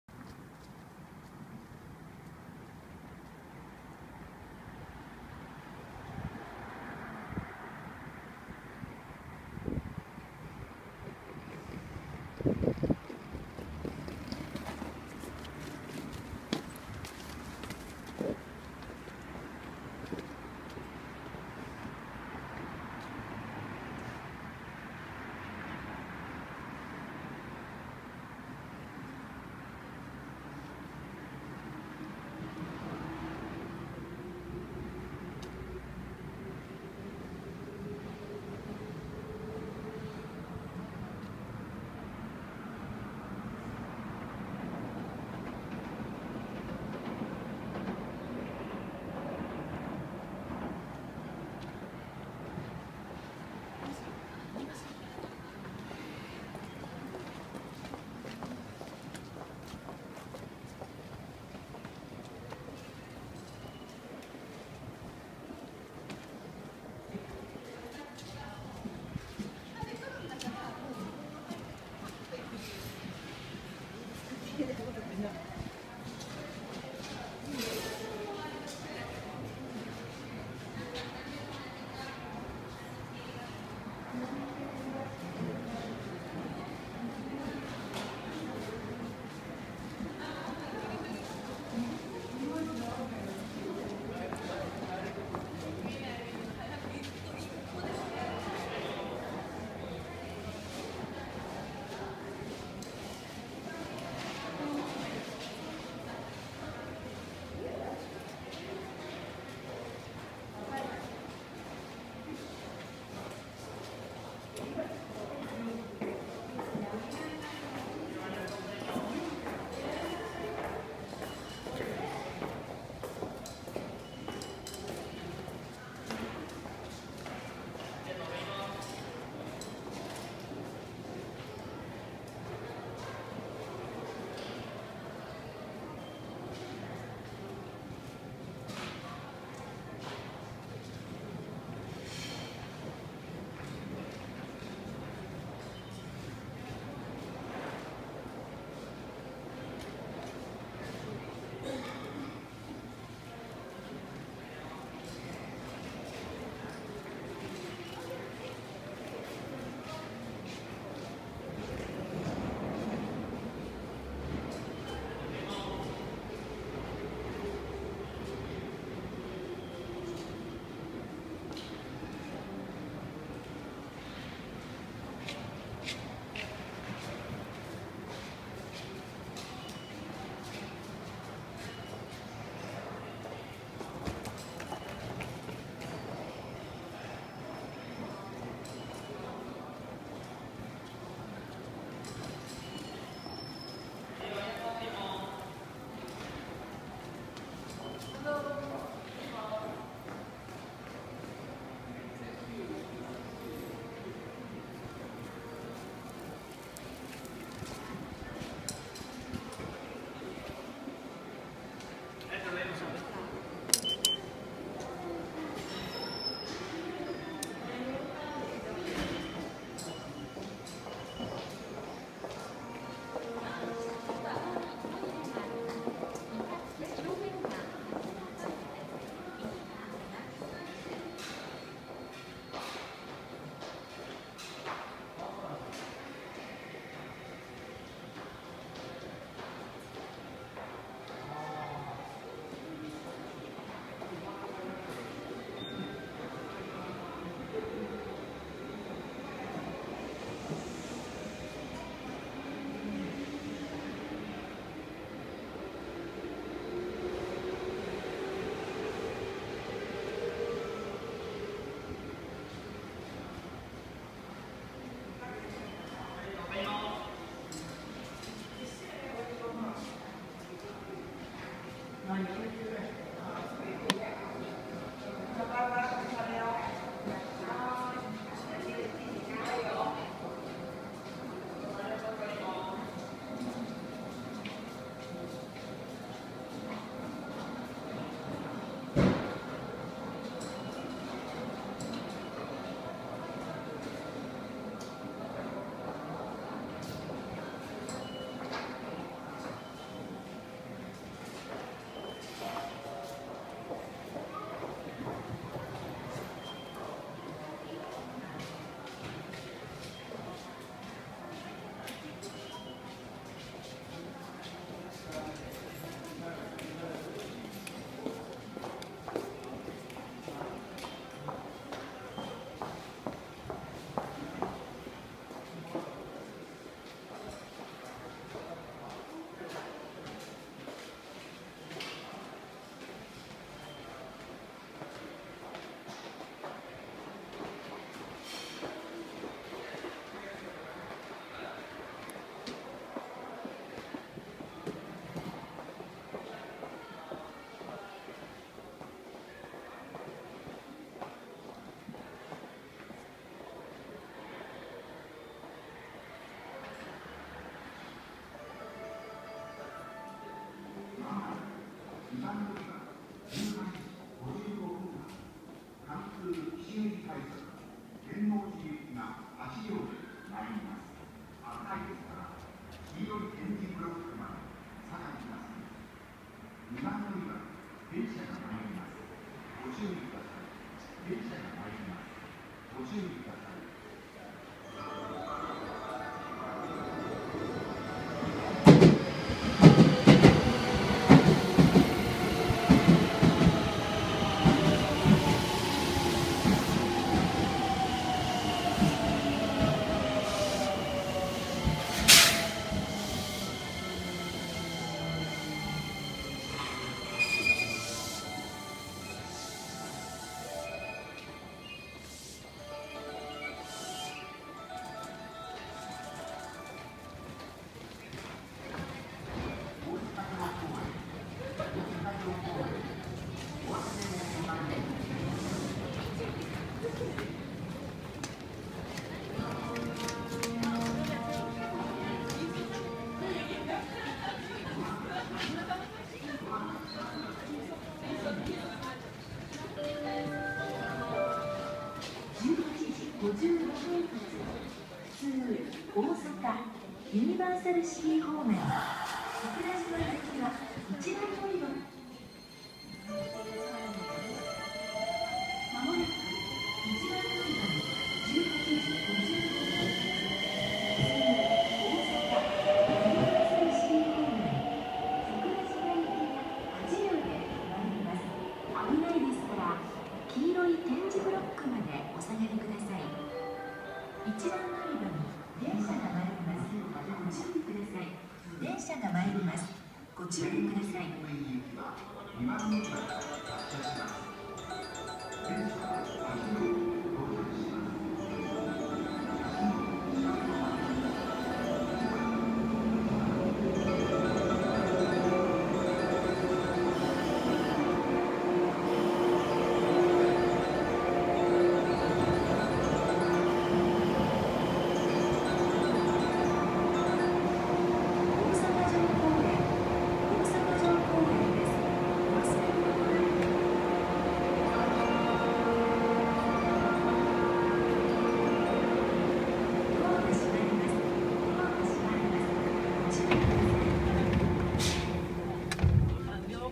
【JR環状線】大阪城公園駅
その名の通り大阪城公園の最寄駅です。大阪城公園は大阪城公園〜森ノ宮の線路西側一帯に広がっていますが天守閣はほぼ中央にあるのでどちらの駅で降りても距離は大して変わりません。大阪城ホールは大阪城公園駅の方が圧倒的に近いです。改札内には大阪城公園の模型が置かれています。また改札外の券売機周辺壁面上部にはタイル絵のようなものがあります。